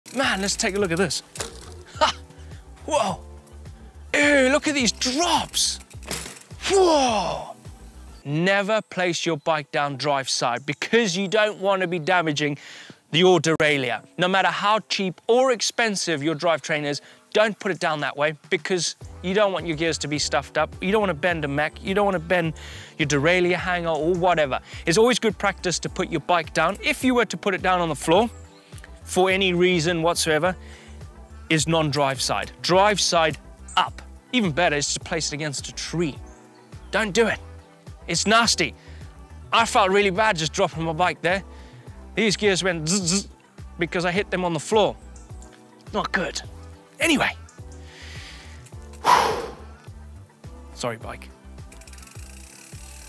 It's VITAL You Do This! 🚲
Taking care of your steed doesn't just stop at giving it a wash! It's essential to look after your derailleur when you're out on the trails to avoid the dreaded bent hanger, or worse...!